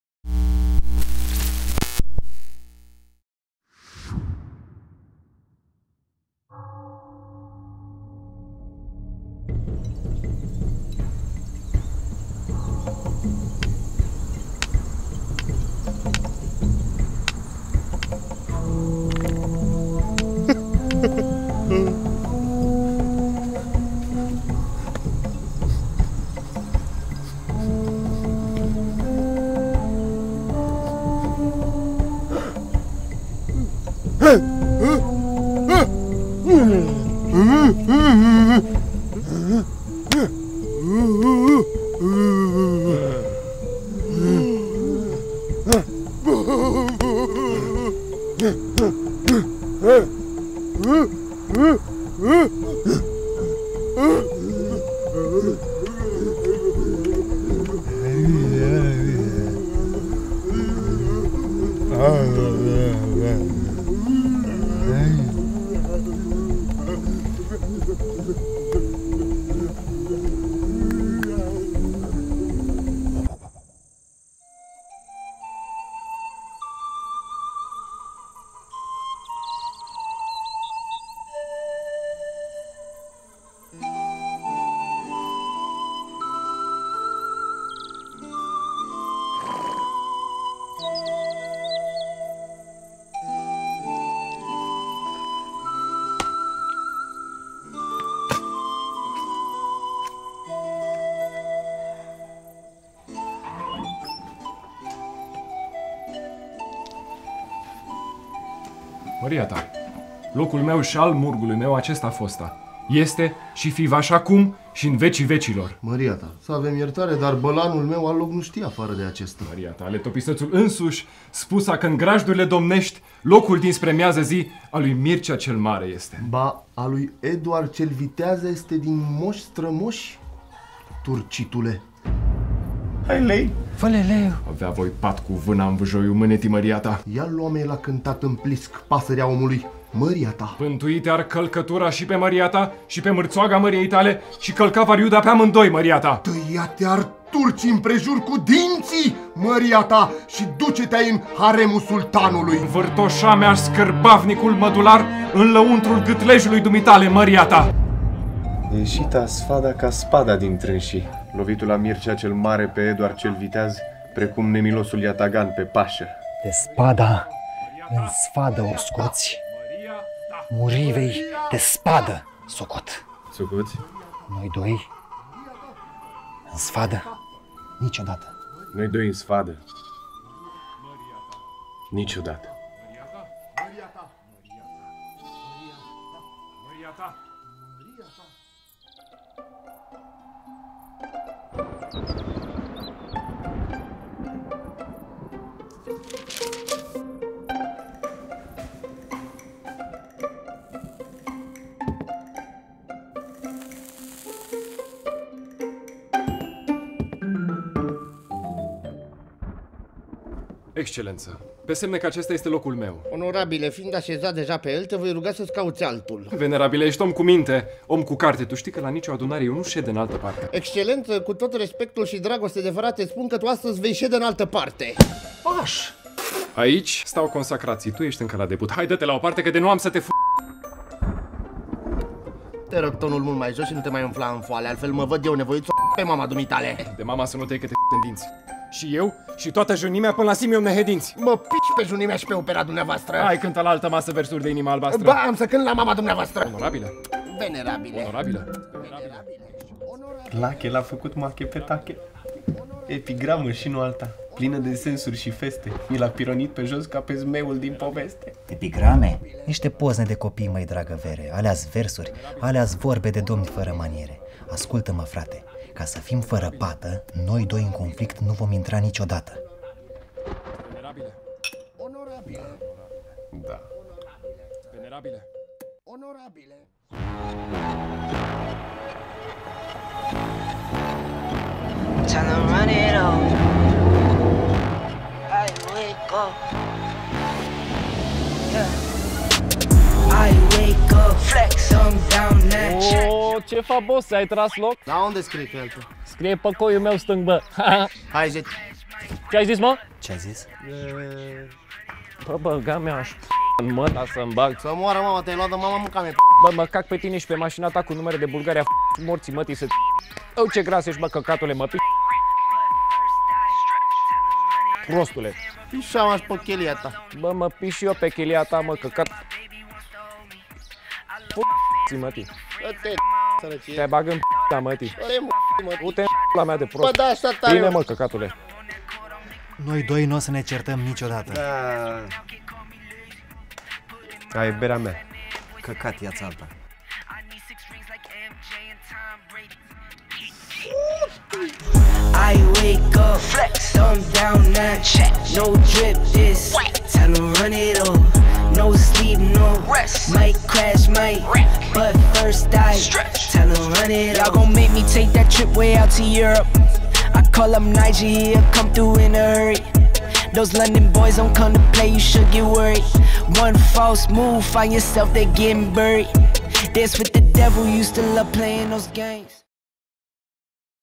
BurBal - O scurtă istorie a înjurăturii
Actori: Ștefan Huluba, Carol Ionescu, Eduard Burghelea, Mircea Alexandru Băluță
Regie: BurBal
Imagine: Adrian Pîrcălabu